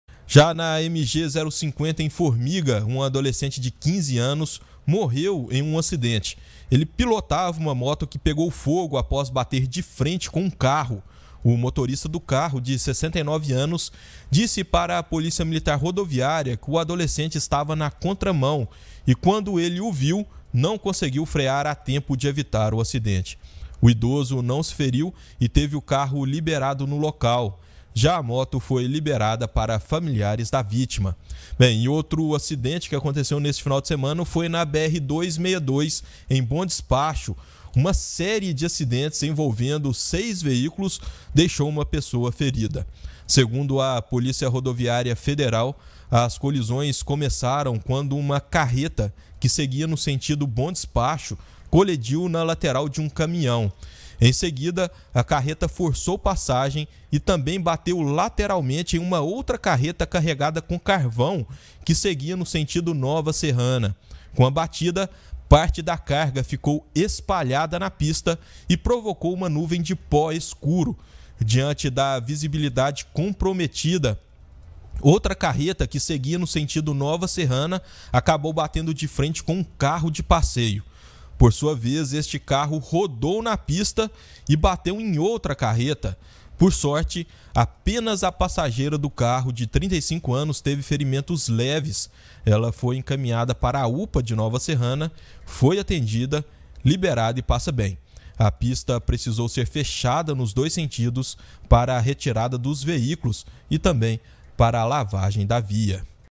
ADOLESCENTE BATE MOTO DE FRENTE COM CARRO E MORRE NA MG-050, EM FORMIGA
| REGIÃO |

NA MG-050, EM FORMIGA, UM ADOLESCENTE DE 15 ANOS MORREU EM UM ACIDENTE. ELE PILOTAVA UMA MOTO QUE PEGOU FOGO APÓS BATER DE FRENTE COM UM CARRO.